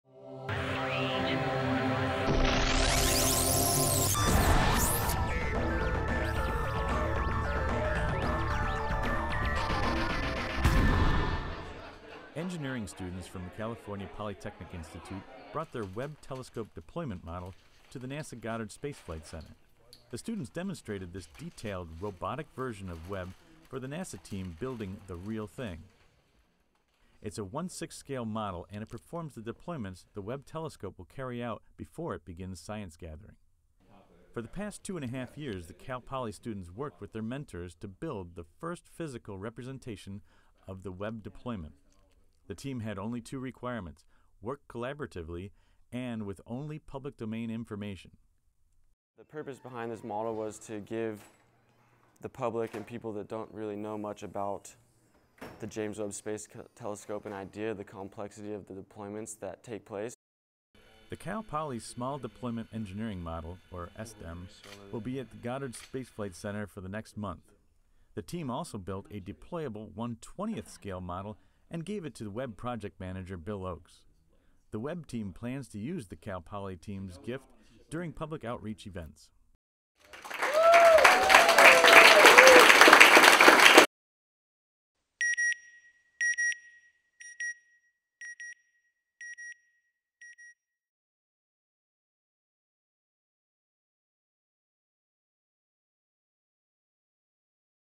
Webb Model Transforms at NASA; Student Engineers Behind the Controls
Engineering students from California Polytechnic Institute brought their Webb Telescope deployment model to NASA Goddard Space Flight Center. The students demonstrated this detailed, robotic version of Webb for the NASA team building the real thing. It's a one - sixth scale model, and it performs the deployments the Webb Telescope will carry out before it begins science gathering.